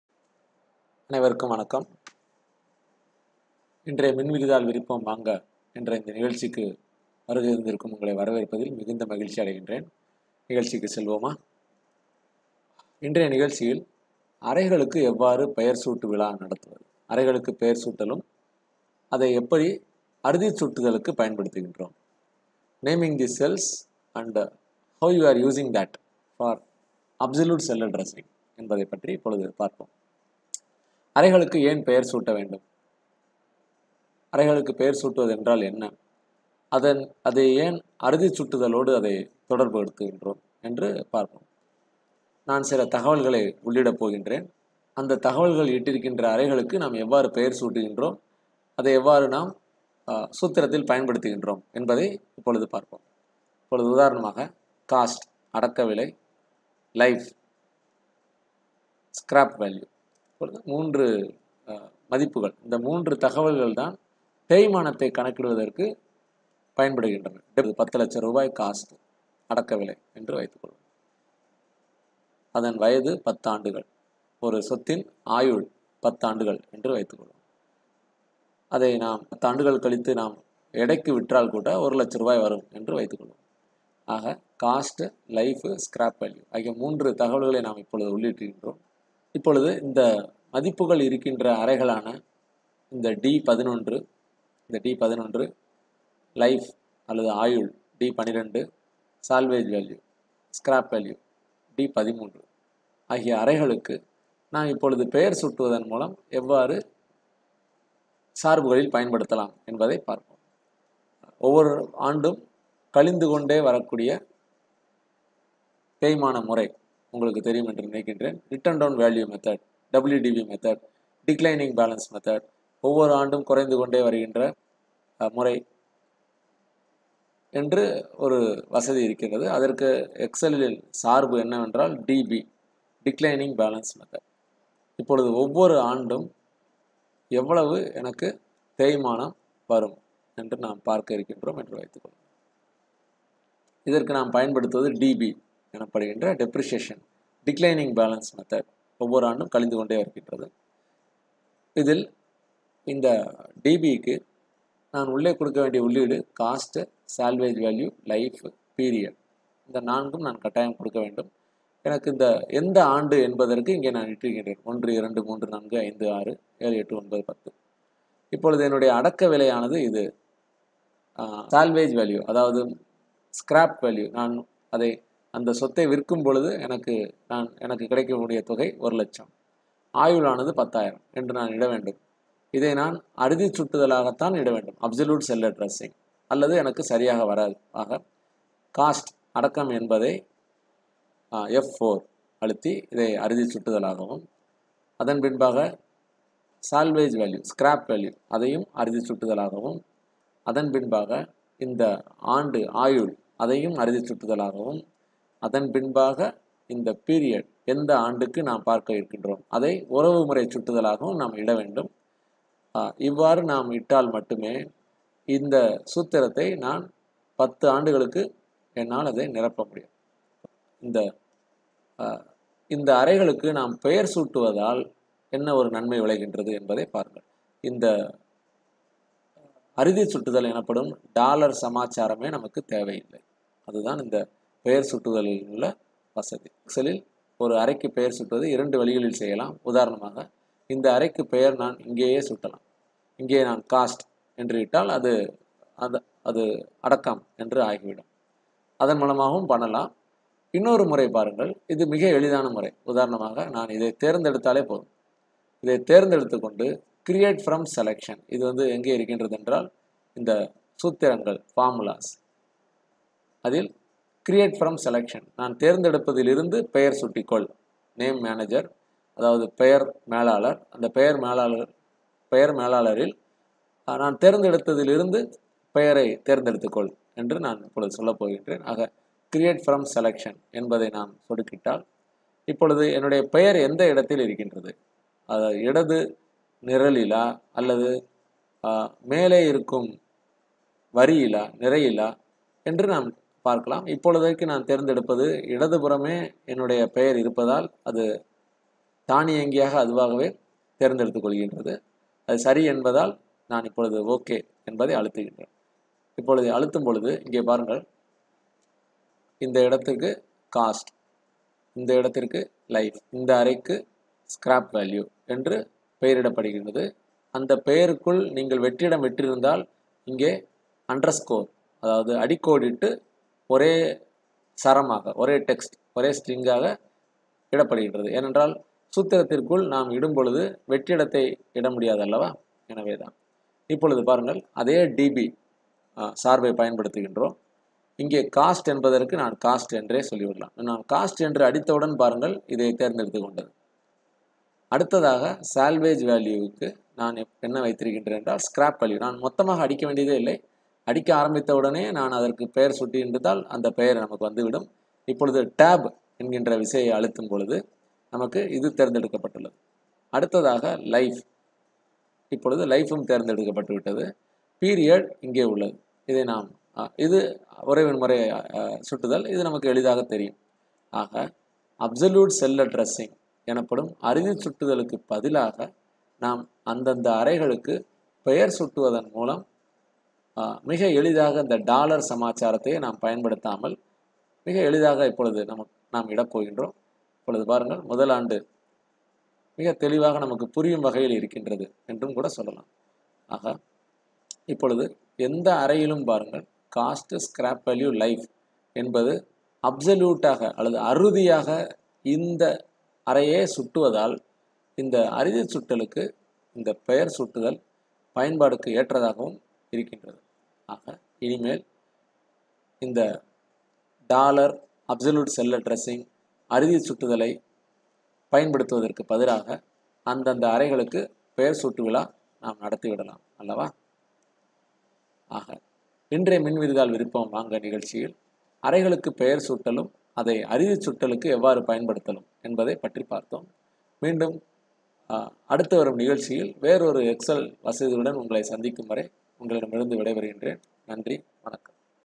அறைகளுக்குப் பெயர் சூட்டலும் அறுதிச் சுட்டலும்
மின்விரிதாள் விரிப்போம் வாங்க - பகுதி - 11 - அறைகளுக்குப் பெயர் சூட்டலும் அறுதிச் சுட்டலும் பற்றி இப்பகுதியில் விளக்கப்படுகின்றது.